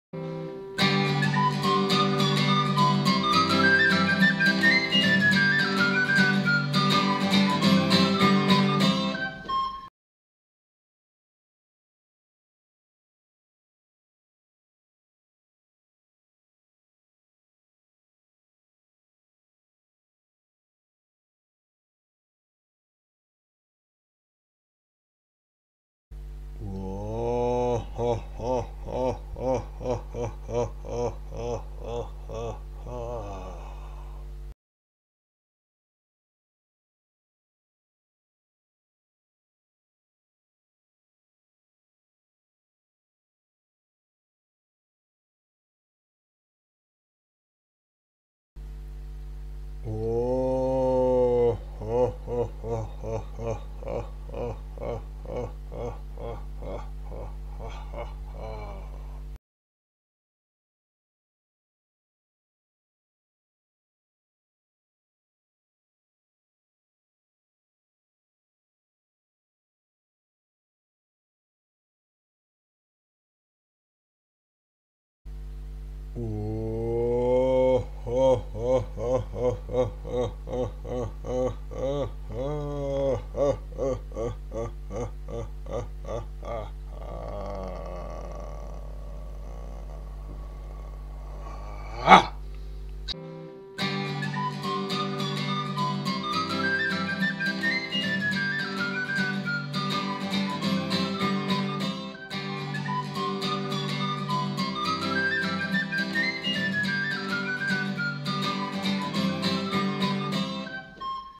Jak se směje medvěd grizzly
Cvičení vyzkoušejte a užijte si ho. Buďte jako tento silný medvěd abstinent! :) Původní hudba: Maestro Vždystřízlivec * : Relaxační nahrávky, smích, cvičení články svépomocné příručky atd.  * Oblíbený čchi-kung * Oblíbená jóga: ww.biharyoga.net * Svépomoc